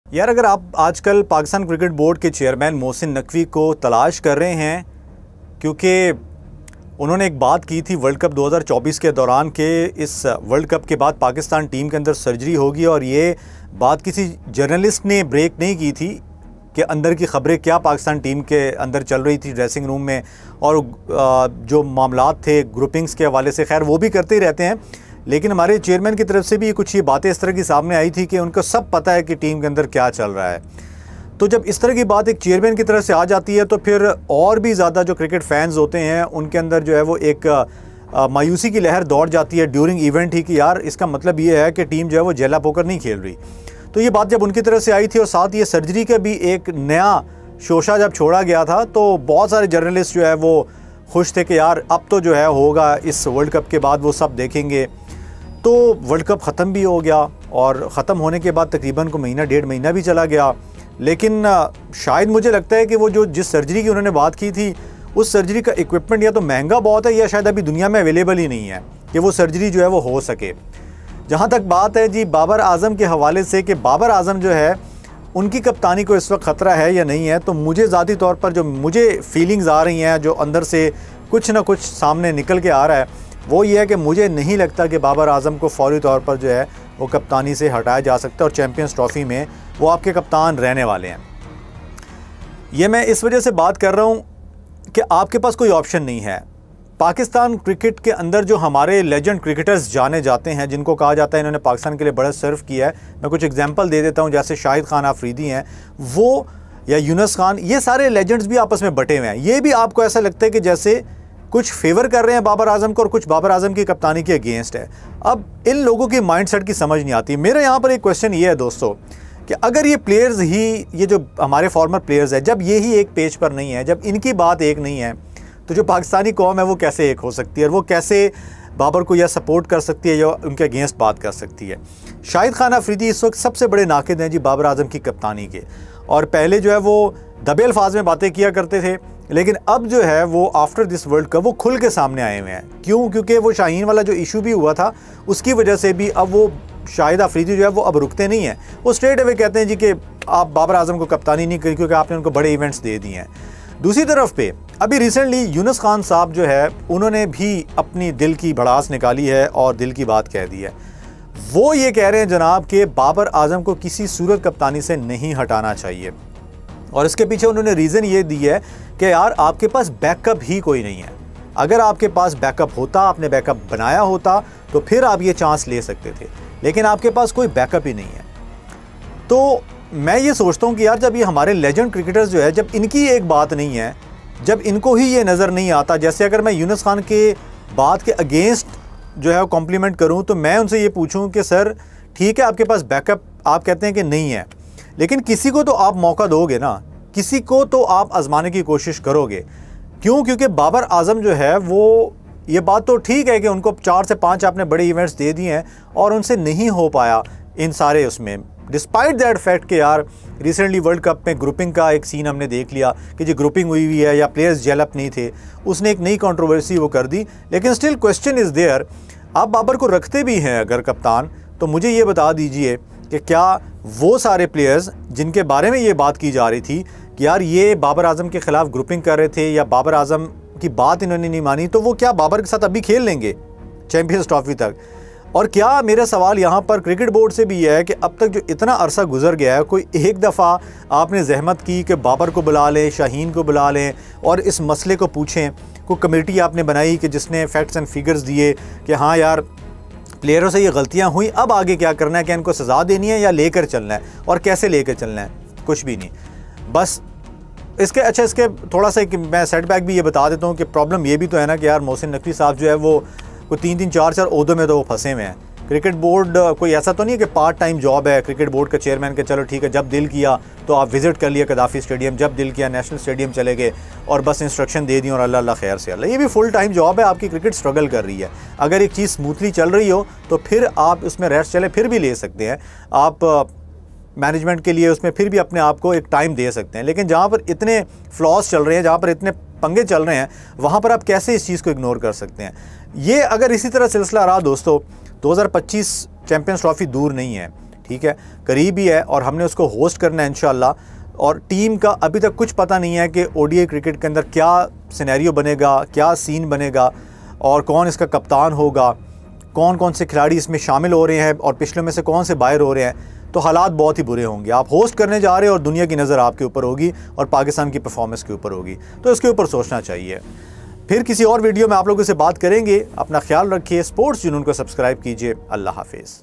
Younus Khan or Shahid Afridi Aamne Saamne | Babar Azam Ki Captaincy Par Sawaliya Nishaan!
After a disappointing World Cup journey, the future of Babar Azam's captaincy is under intense scrutiny. Former Pakistani cricketers are divided in their opinions, with some supporting Babar Azam and others advocating for a leadership change. Dive into this heated debate and explore the possible outcomes for Pakistan cricket.